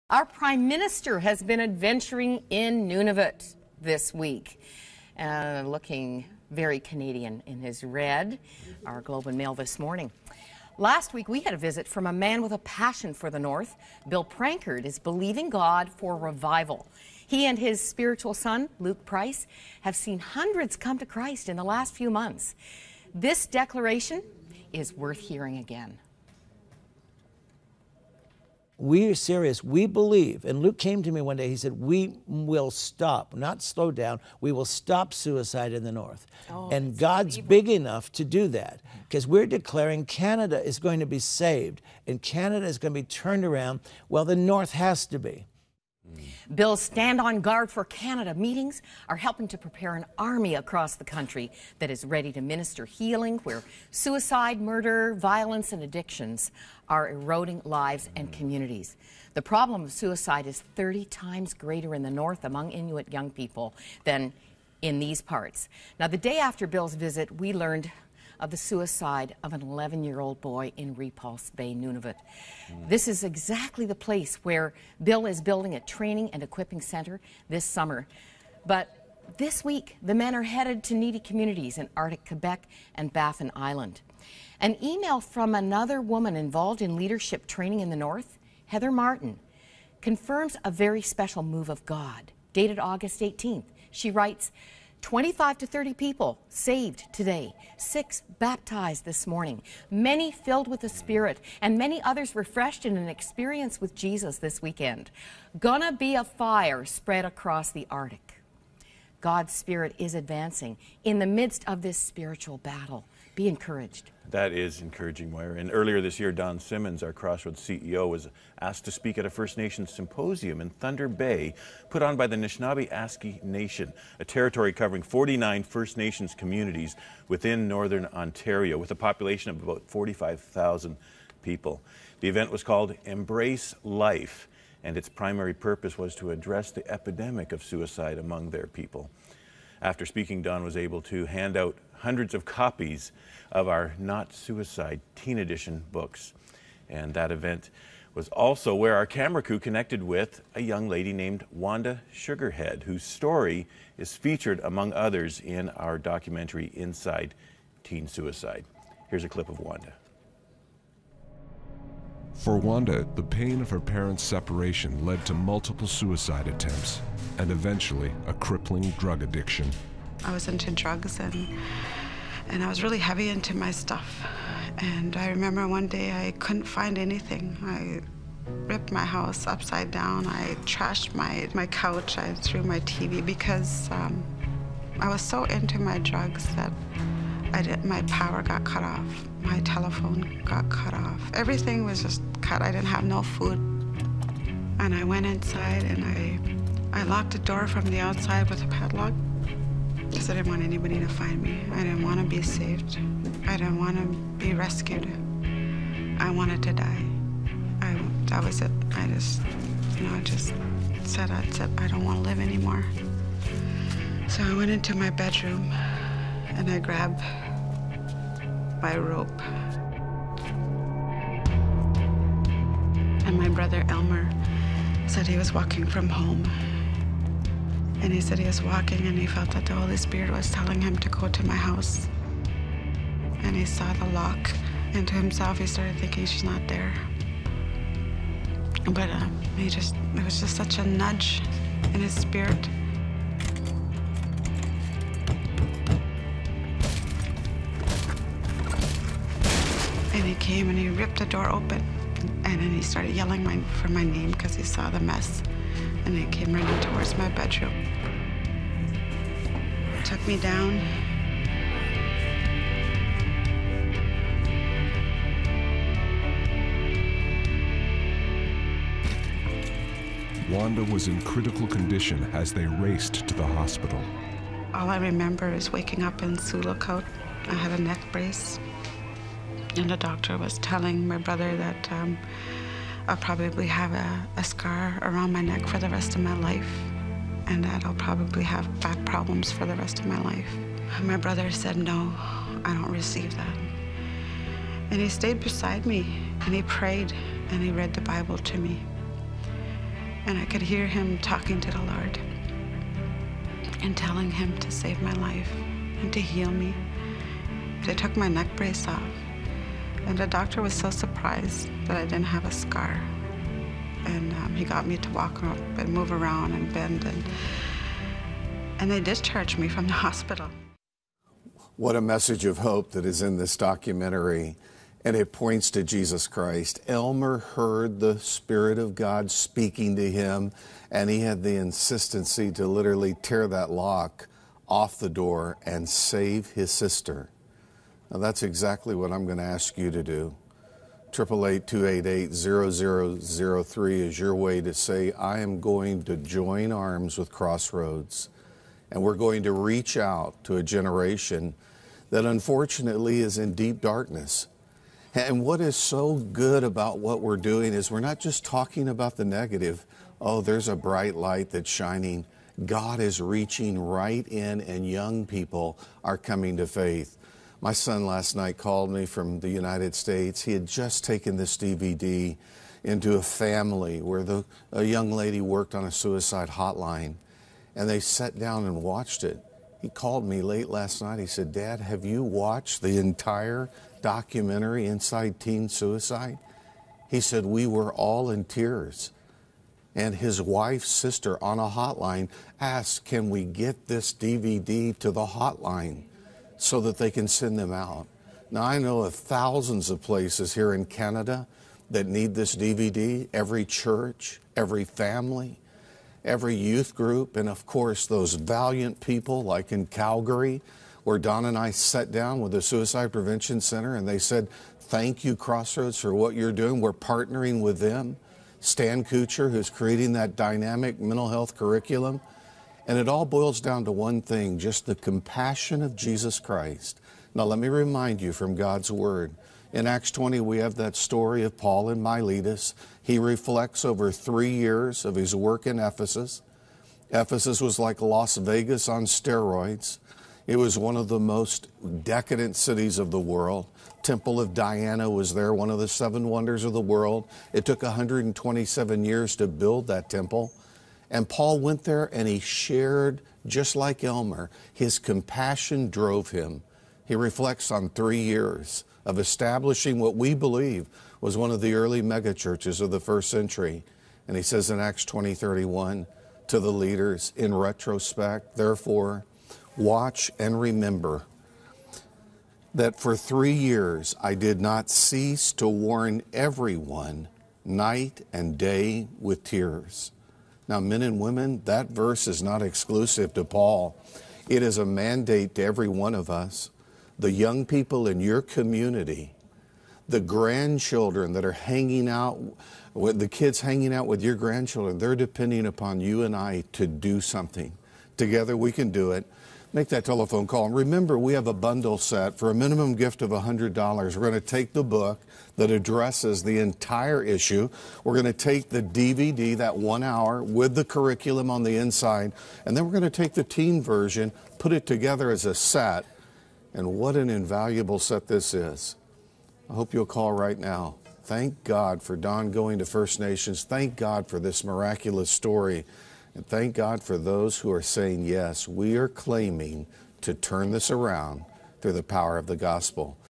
Facing Suicide in the North -- Wanda Sugarhead
Wanda Sugarhead talks about her attempted suicide.